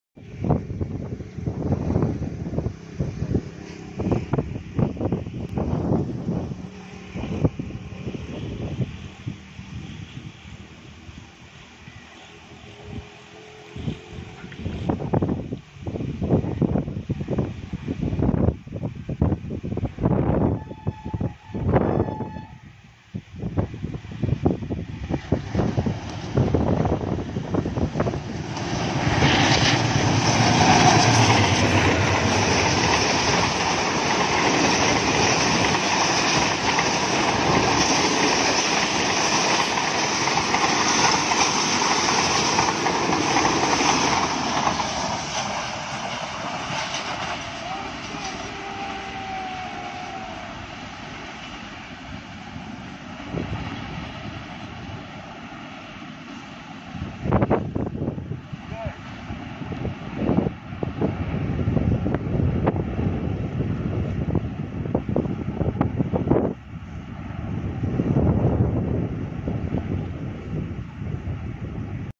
Pak Business Express crossing Link Canal at High Speed || Raising Dust || Tiger AGE-30 on the lead
33 Up Pak Business Express crossing link canal near changa manga